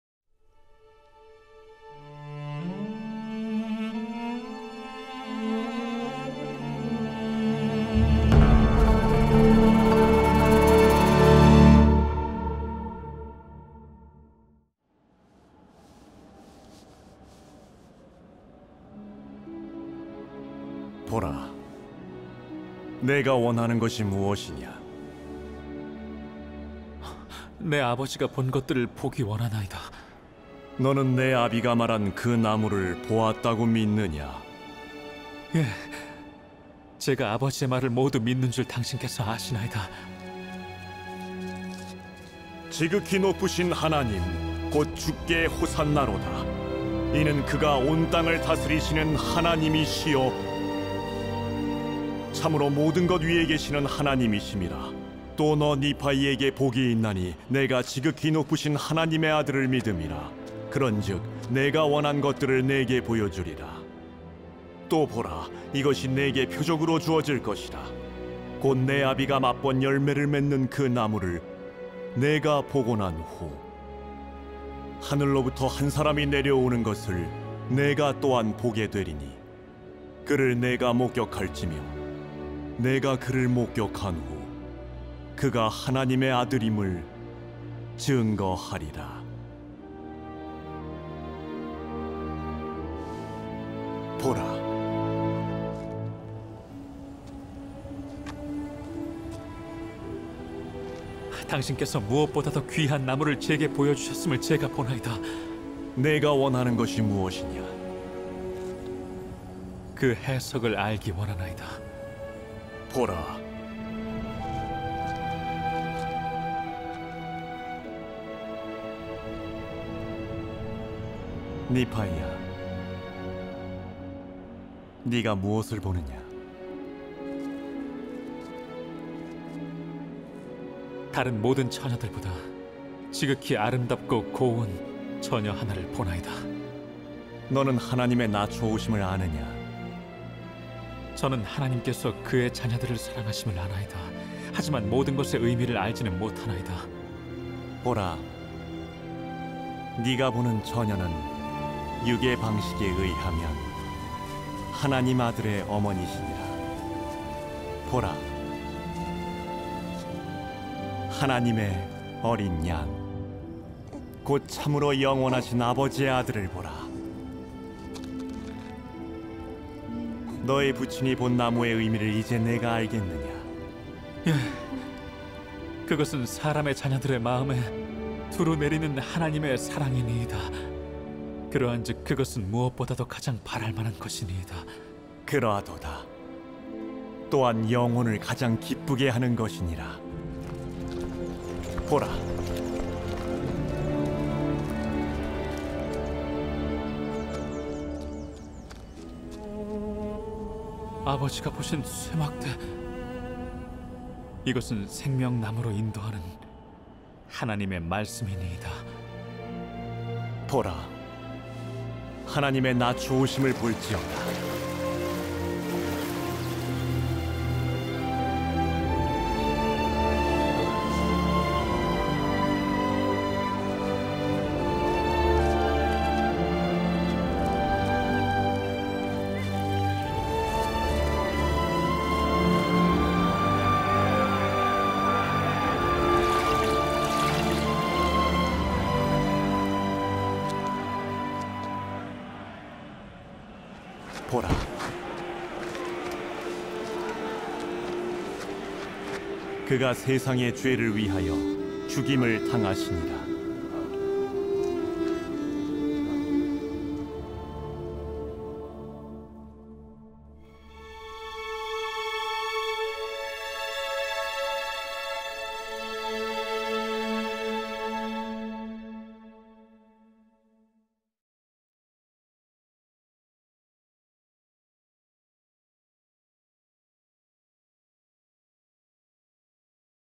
니파이가 예수 그리스도의 탄생과 성역을 보다 | 니파이전서 11장
니파이가 기도를 드리고 천사들의 방문을 받는다. 니파이는 하나님의 아들의 시현을 보고 그분이 하나님의 어린 양으로서 베푸시는 성역을 보다. 니파이는 처녀 어머니를 보고 인간의 자녀를 위한 하나님의 사랑이라고 해석하다.

1 이는 이렇게 되었음이니 내가 나의 부친이 보신 것들을 심히 알기 원한 후에, 주께서 그것들을 나에게도 알려 주실 수 있으리라고 믿으며 내 마음으로 깊이 생각하며 앉아 있었을 때, 나는 주의 영에게 붙들리어, 내가 전에 한 번도 본 적이 없으며, 전에 한 번도 발을 딛어 본 적이 없는 심히 높은 산으로 이끌려 갔느니라.

2 그리고 영이 내게 이르시되, 보라, 네가 원하는 것이 무엇이냐?

3 이에 내가 이르되, 나의 아버지가 본 것들을 보기 원하나이다 하였더라.

4 그리고 영이 내게 이르시되, 너는 네 아비가 말한 그 나무를 보았다고 믿느냐?

5 이에 내가 이르되, 그러하오이다. 내가 내 아버지의 말을 모두 믿는 줄 당신께서 아시나이다 하였더라.

6 그리고 내가 이러한 말들을 하고 나자, 영이 큰 음성으로 외쳐 이르시되, 지극히 높으신 하나님, 곧 주께 호산나로다. 이는 그가 온 땅을 다스리시는 하나님이시요, 참으로 모든 것 위에 계시는 하나님이심이라. 또 너 니파이에게 복이 있나니 네가 지극히 높으신 하나님의 아들을 믿음이라. 그런즉, 네가 원한 것들을 네게 보여 주리라.

7 또 보라, 이것이 네게 표적으로 주어질 것이라. 곧 네 아비가 맛 본 열매를 맺는 그 나무를 네가 보고 난 후, 하늘로부터 한 사람이 내려오는 것을 네가 또한 보게 되리니, 그를 네가 목격할지며 네가 그를 목격한 후, 그가 하나님의 아들임을 증거하리라 하시더라.

8 이에 이렇게 되었나니 영이 내게 일러, 보라 하시기로 내가 보고는 나무 한 그루를 보았나니, 그것은 나의 부친이 보신 나무와 흡사하더라. 또 그 아름다움은 모든 아름다움을 훨씬 능가하여 참으로 어떤 아름다움보다도 더욱 뛰어났으며, 그 희기는 쌓인 눈의 희기보다 더 희더라.

9 또 이렇게 되었나니 내가 나무를 본 후에 영께 이르되, 당신께서 무엇보다도 귀한 나무를 내게 보여 주셨음을 내가 보나이다 하였더라.

10 그리고 그가 내게 이르시되, 네가 원하는 것이 무엇이냐?

11 이에 내가 그에게 이르되, 그 해석을 알기 원하나이다 하였으니, 이는 내가 사람이 말하듯 그에게 말하였음이라. 이는 그가 사람의 형상을 하고 계심을 내가 보았음이어니와, 그럼에도 불구하고 나는 그가 주의 영인 줄을 알았고, 또 그는 사람이 다른 사람과 말하듯 나에게 말씀하셨느니라.

12 이에 이렇게 되었나니 그가 내게 일러, 보라 하시기로 내가 마치 그를 쳐다보려는 듯 보았으나, 내가 그를 보지 못하였으니 이는 그가 내 앞에서 떠나가셨음이더라.

13 이에 이렇게 되었나니 내가 보고 큰 성 예루살렘과 또한 다른 성읍들을 보았고, 나는 또 나사렛 성을 보았으며 나사렛 성 안에 한 처녀를 보았나니 그 처녀는 심히 곱고 희더라.

14 또 이렇게 되었나니 내가 보니, 하늘들이 열리고 한 천사가 내려와 내 앞에 서서 내게 이르되, 니파이야 네가 무엇을 보느냐?

15 이에 내가 그에게 말하기를, 다른 모든 처녀들보다 뛰어나게 지극히 아름답고 고운 처녀 하나를 보나이다 하였더라.

16 또 그가 내게 이르기를 너는 하나님의 낮추어 오심을 아느냐?

17 이에 내가 그에게 말하기를, 나는 하나님께서 그의 자녀들을 사랑하심을 알고 있나이다. 그러할지라도 모든 것의 의미를 알지는 못하나이다 하였더라.

18 또 그가 내게 이르기를, 보라, 네가 보는 처녀는 육의 방식에 의하면 하나님 아들의 어머니시니라 하더라.

19 또 이렇게 되었나니 나는 그 처녀가 영에 이끌려 감을 보았으며, 얼마 동안 그 처녀가 영에 이끌려 가 있은 후에 천사가 내게 말하여 이르되, 보라 하였느니라.

20 이에 내가 보고 그 처녀를 다시 보았더니 한 어린아이를 팔에 안고 있더라.

21 또 천사가 내게 이르되, 하나님의 어린 양 곧 참으로 영원하신 아버지의 아들을 보라! 너의 부친이 본 나무의 의미를 이제 네가 알겠느냐?

22 이에 내가 그에게 대답하여 이르되, 그러하오이다. 그것은 사람의 자녀들의 마음에 두루 내리는 하나님의 사랑이니이다. 그러한즉 그것은 무엇보다도 가장 바랄만한 것이니이다 하였느니라.

23 또 그가 내게 일러 이르되, 그러하도다. 또한 영혼을 가장 기쁘게 하는 것이니라 하더라.

24 또 그가 이 말들을 하고 난 후, 나에게 일러, 보라 하기로, 내가 보고는 하나님의 아들이 사람의 자녀들 가운데 나아가시는 것을 보았느니라. 또 내가 본즉, 많은 자들이 그의 발에 엎드려 그를 경배하더라.

25 또 이렇게 되었나니 나는 나의 부친이 보신 쇠막대가 생수의 근원, 곧 생명나무로 인도하는 하나님의 말씀임을 알았나니, 이 물은 하나님의 사랑의 표현이니라. 또 나는 생명나무가 하나님의 사랑의 표현임도 알았느니라.

26 또 천사가 다시 내게 이르기를, 보라 그리하여 하나님의 낮추어 오심을 볼 지어다!

27 이에 내가 보고는 나의 부친이 말씀하신 세상의 구속주를 보았고, 또한 그의 앞에서 길을 예비할 선지자도 보았나니, 하나님의 어린 양이 나아가사 그에게 침례를 받으시더라. 또 그가 침례를 받으신 후 내가 보니 하늘들이 열리고, 성신이 하늘에서 내려와, 비둘기의 형상으로 그의 위에 머무르더라.

28 또 내가 보니 그가 능력과 큰 영광으로 나아가 백성들에게 성역을 베푸시매, 허다한 무리가 그의 말을 들으려고 함께 모였더라. 또 내가 보니, 그들이 그를 그들 가운데서 쫓아 내더라.

29 또 나는 다른 열둘이 그를 따르는 것도 보았느니라. 그리고 이렇게 되었나니 그들이 나의 면전에서 영에 이끌려 가더니 보이지 아니하더라.

30 또 이렇게 되었나니 천사가 다시 내게 일러 이르되, 보라 하기로 내가 보고 하늘들이 다시 열린 것을 보았고, 또 천사들이 사람의 자녀들 위에 내려오는 것을 보았나니 그들이 사람의 자녀들에게 성역을 베풀더라.

31 또 그가 다시 내게 일러 이르되, 보라 하기로 내가 보니 하나님의 어린 양이 사람의 자녀들 가운데 나아가시더라. 또 나는 병든 자들과 온갖 종류의 질병으로 또 마귀들과 더러운 귀신들로 고통받는 허다한 무리를 보았나니, 천사가 이 모든 것을 나에게 말하고 보여 주었느니라. 이에 그들이 하나님의 어린 양의 권능으로 고침을 받고 마귀들과 더러운 귀신들이 쫓겨나더라.

32 또 이렇게 되었나니 천사가 다시 내게 일러 이르되, 보라 하기로 내가 보고는 하나님의 어린 양을 보았나니 그가 사람들에게 잡히셨더라. 참으로 영원하신 하나님의 아들이 세상에게 심판을 받으시매 내가 보고 증거하였노라.

33 또 나 니파이가 보니, 그가 십자가 위에 들리어 세상의 죄를 위하여 죽임을 당하시더라.

34 또 그가 죽임을 당하신 후에 내가 본즉, 땅의 허다한 무리가 어린 양의 사도들을 대적하여 싸우려고 함께 모였더라. 어린 양의 사도라 함은 주의 천사가 열둘을 이같이 일컬었음이라.

35 또 땅의 허다한 무리가 함께 모였는데 내가 보니, 그들이 나의 부친이 보신 것과 흡사한 한 크고 넓은 건물 안에 있더라. 주의 천사가 다시 내게 일러 이르되, 세상과 그 지혜를 보라. 참으로, 보라 이스라엘의 집이 어린 양의 열두 사도를 대적하여 싸우려고 함께 모였도다 하더라.

36 또 이렇게 되었나니 내가 보고 증거하였으되, 그 크고 넓은 건물은 세상의 교만이요, 또 그것이 무너지니, 그 무너짐이 심히 크더라. 이에 주의 천사가 다시 내게 일러 이르되, 어린 양의 열두 사도를 대적하여 싸울 모든 나라와 족속과 방언과 백성의 멸망이 이러하리라 하더라.